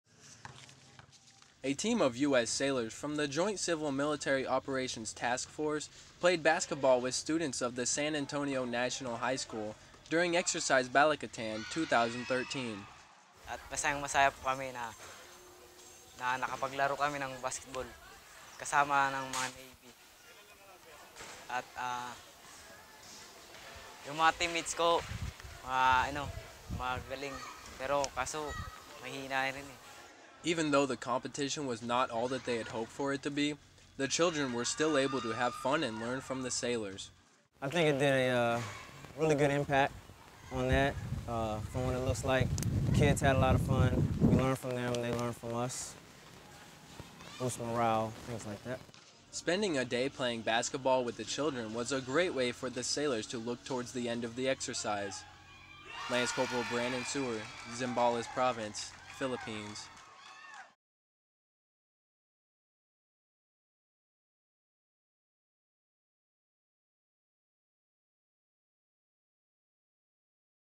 U.S. Sailors bond with children through basketball
U.S. sailors from the Joint Civil Military Operations Task Force (JCMOTF) participated in a basketball game with students of the San Antonio National High School during Exercise Balikatan 2013. Balikatan is an annual Philippine-U.S. bilateral exercise. Humanitarian assistance and training activities enable the Philippine and American service members to build lasting relationships, train together and provide assistance in communities where the need is the greatest. Lance Cpl. Brandon Suhr shows us the highlight reel.